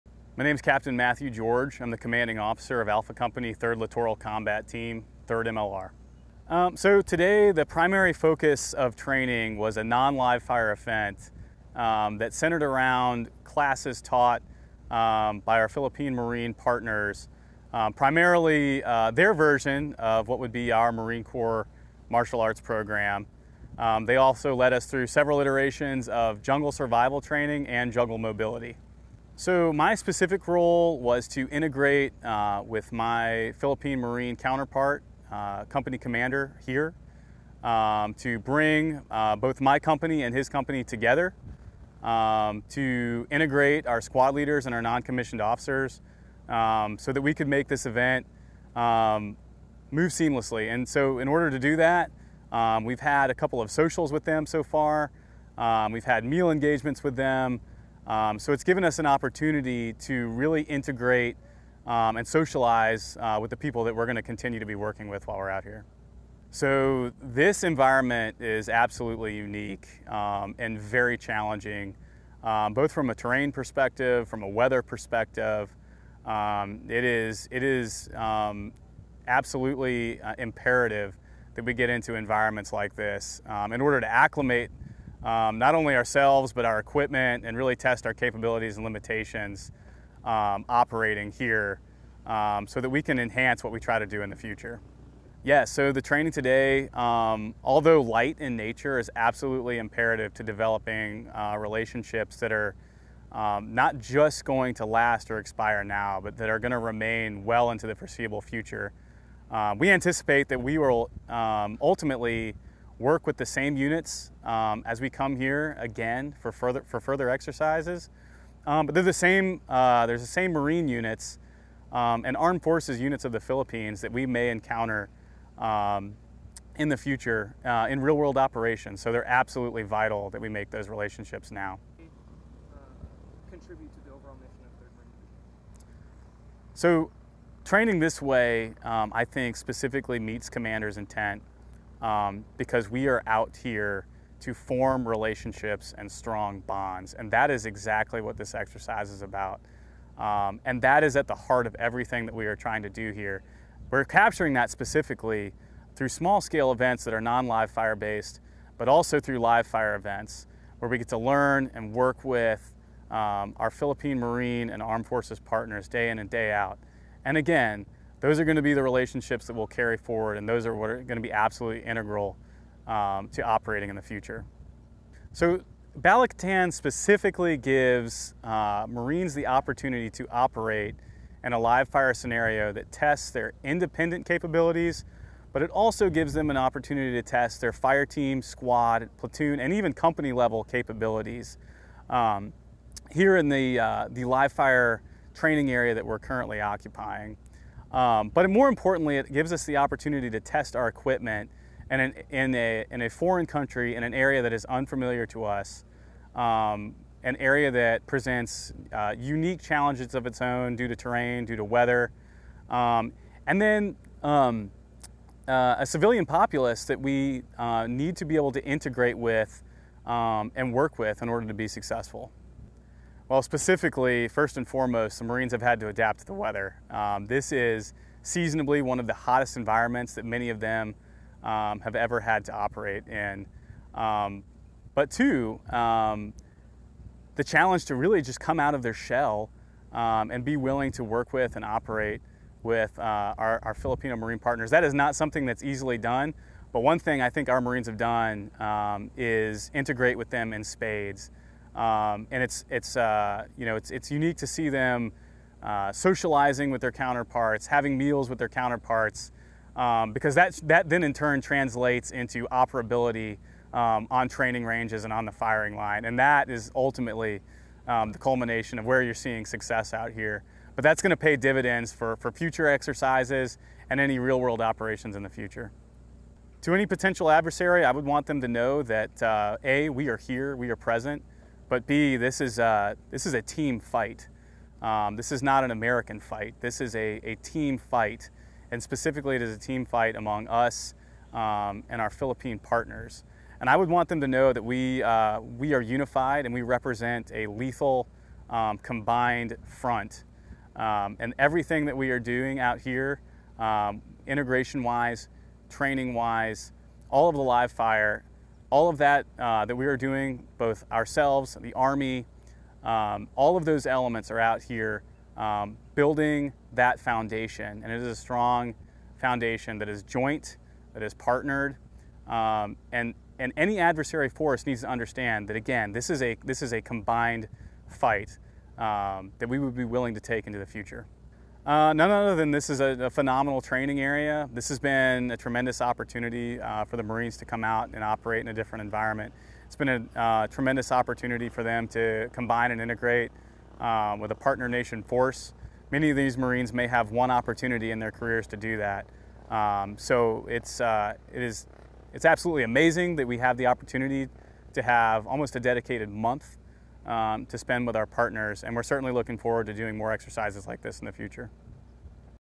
Navy Captain's Incredible Story: Balikatan 23 & Why This Change Everything!
U.S. Marine Corps Capt. Matthew George, company commander for Alpha Company, 3d Littoral Combat Team, 3d Marine Littoral Regiment, 3d Marine Division, speaks on, Balikatan 23, in Santa Juliana, Philippines, April 13, 2023.

Balikatan 23 | 3d LCT A Co. Company Commander Capt. Matthew George Interview
PHILIPPINES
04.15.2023

 Balikatan 2023 is the 38th iteration of the annual bilateral exercise between the Armed Forces of the Philippines and the U.S. military. The exercise includes three weeks of training focused on amphibious operations, command and control, humanitarian assistance, urban operations and counterterrorism skills throughout northern and western Luzon. Coastal defense training figures prominently in the Balikatan 23 training schedule. (U.S. Marine Corps video by Lance Cpl. Noah Masog)

Film Credits: Video by Lance Cpl. Noah Masog
Exercise Balikatan